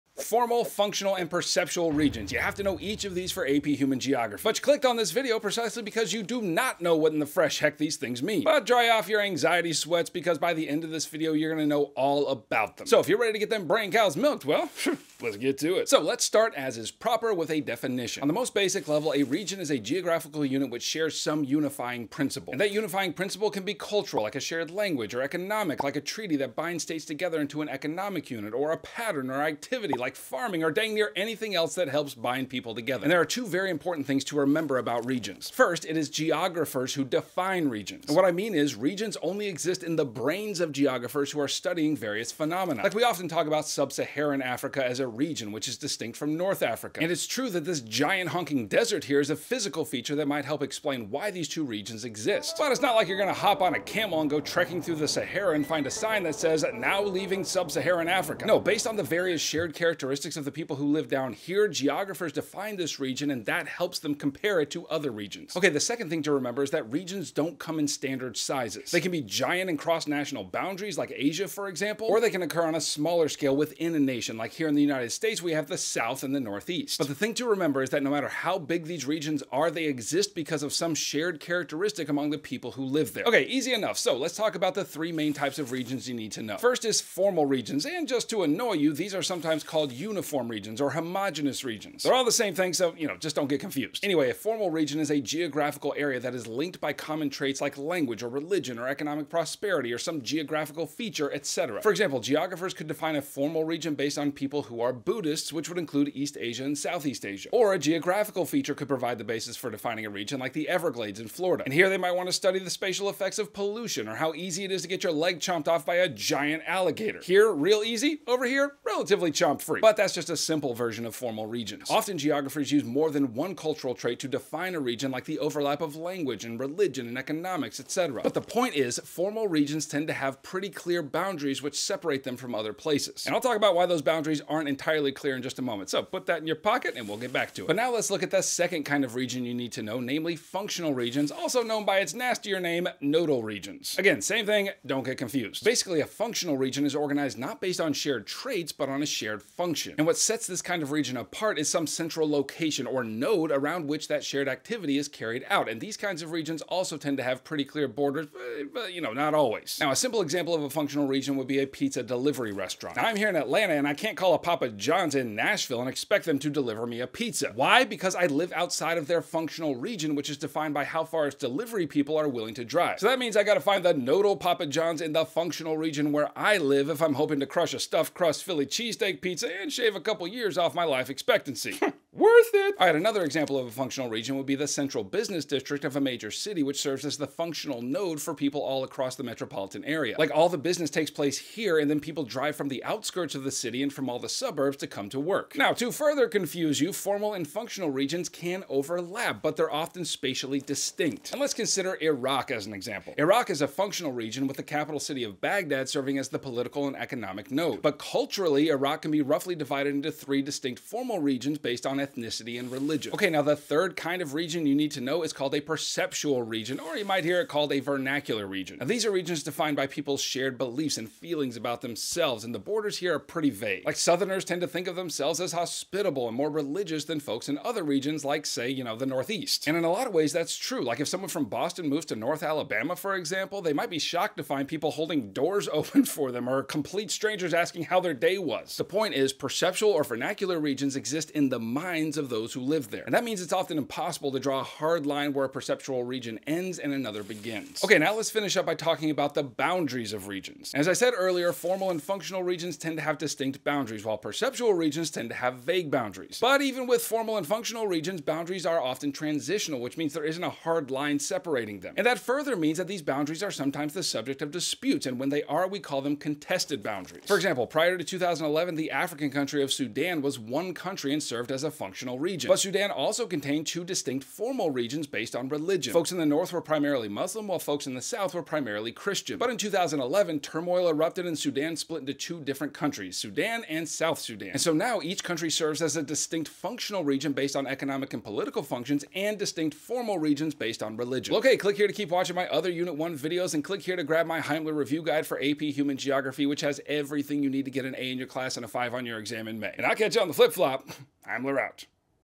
Understanding REGIONAL ANALYSIS [AP Human Geography Review—Unit 1 Topic 7]
In this video Heimler covers everything you need to know about AP Human Geography Unit 1 Topic 7 (1.7) which is all about regional analysis including formal, functional, and perceptual regions.